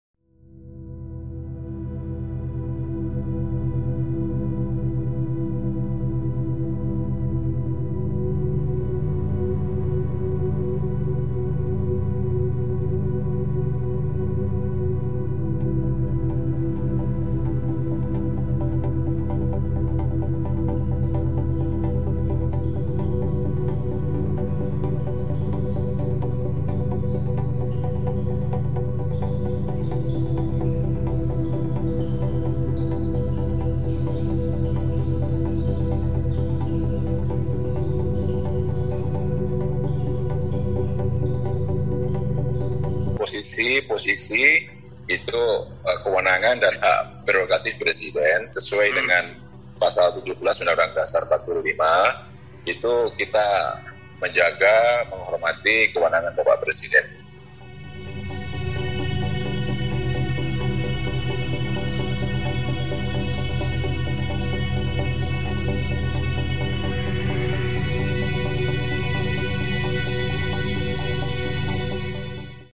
Isu PAN Dapat Jatah Kursi Menteri, Ini Jawaban Waketum PAN
Isu tentang ketua Umum PAN Zulkifli Hasan mendapatkan jatah kursi menteri pun santer dibicarakan. Isu ini santer dibicarakan usai PAN bergabung dengan koalisi Parpol pendukung Jokowi. Atas isu tersebut, Wakil Ketua Umum DPP Partai Amanat Nasional (PAN) Viva Yoga Mauladi menegaskan bahwa partainya akan menyerahkan sepenuhnya kepada Presiden Joko Widodo terkait posisi PAN di Kabinet Indonesia Maju.